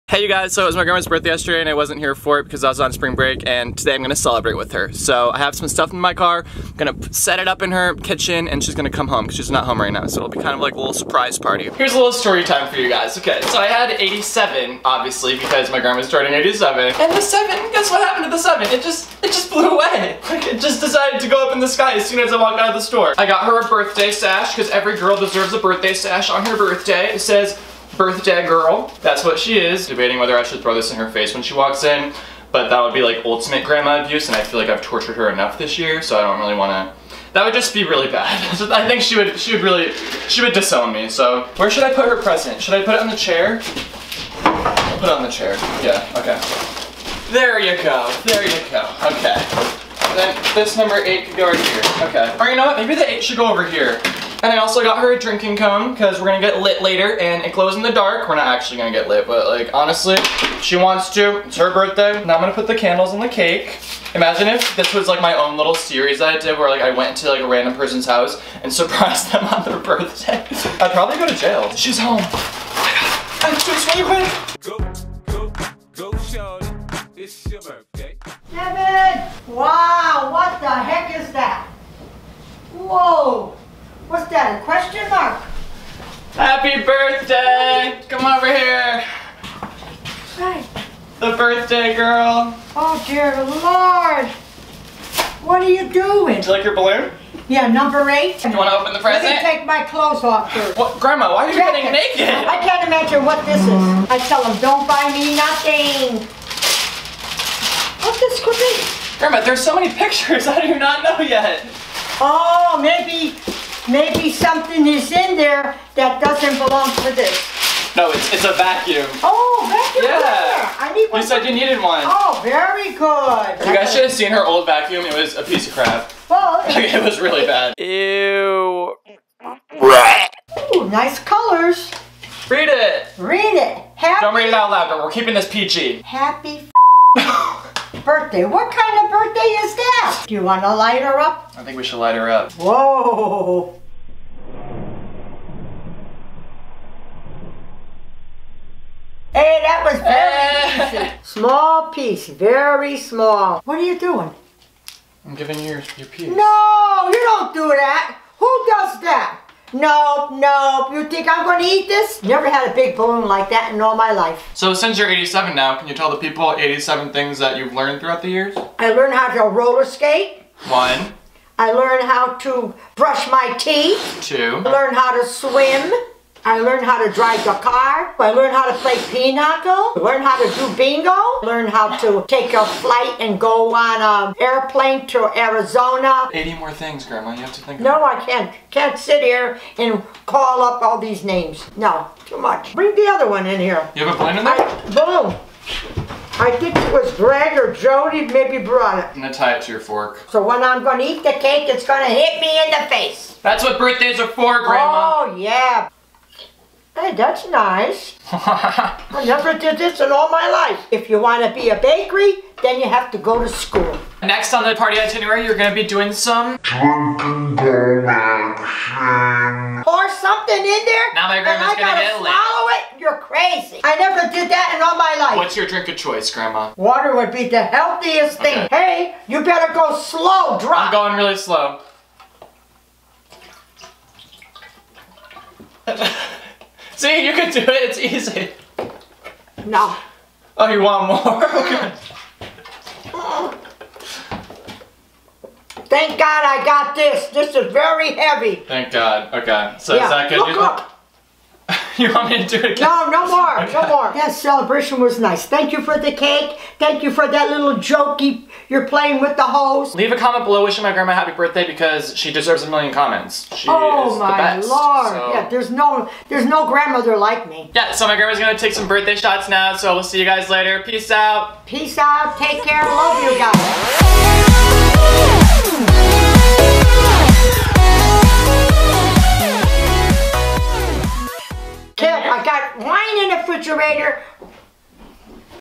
GRANDMA LILL CELEBRATES HER BIRTHDAY
I throw a little surprise party for my grandma and make her drink out of a drinking funnel.